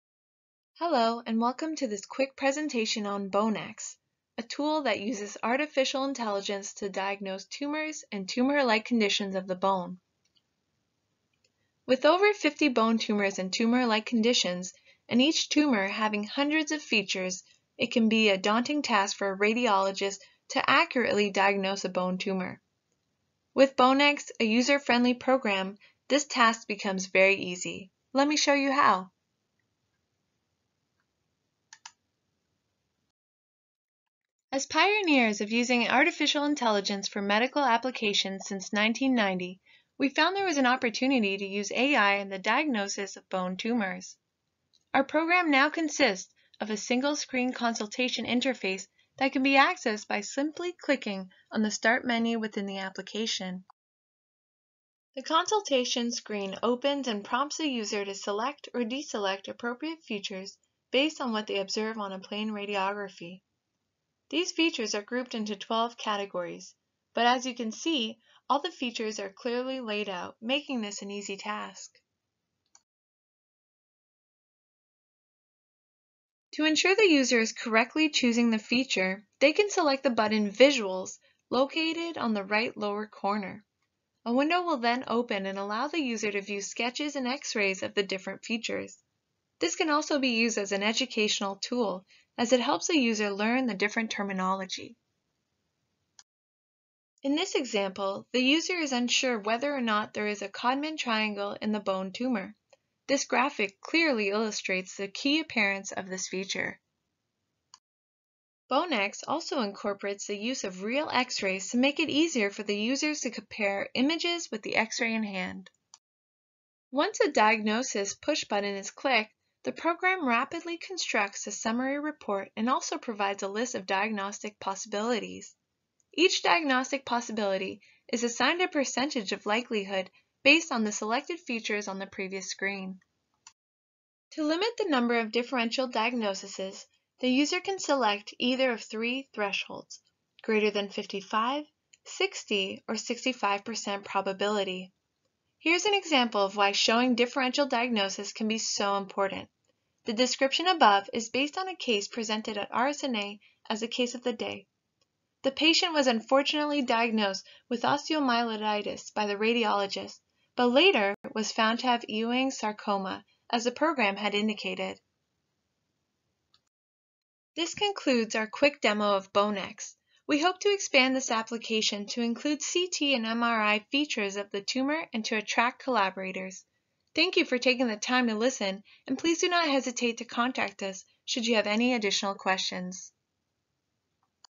Artificial Intelligence (AI) for Medical Education : Bone Tumors
An educational  knowledge-based decision support guidance for bone tumors using X-ray.